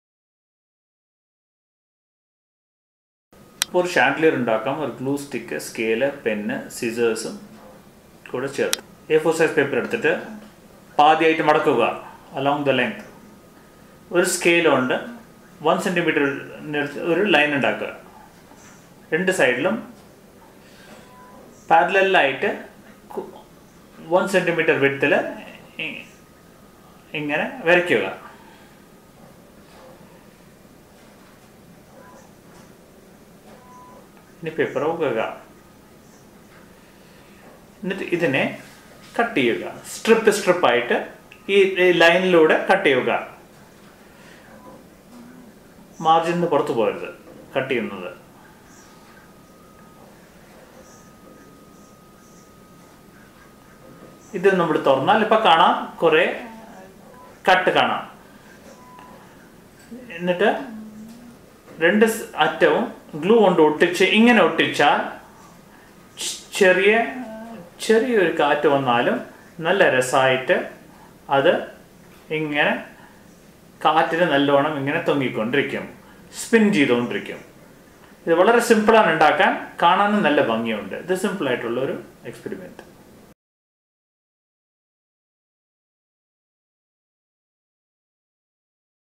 PAPER CHANDELIER | Malayalam
DUBBED IN MALAYALAM BY MR. SUNIL KUMAR

You can make this very pretty chandelier using A-4 size paper, scale, scissors, glue stick and a pen. Fold the A-4 paper in half along the width. Draw a 1-cm line along one open long edge. Then draw parallel lines all along the length of the paper. These lines will be about 1-cm apart. You can open the paper and see that these lines are actually very simple to make. Now cut from the FOLD to the MARGIN line. Do not go beyond the margin line. Cut all the parallel lines. Open the paper to see a series of parallel cut lines. Apply glue to two opposite corners and stick them together. And this step will make the Chandelier. The Chandelier will sway on the slightest breeze. On hanging by a thin thread the Chandelier will spin very elegantly in the breeze.

Dubbed in Malayalam by Aravind Raj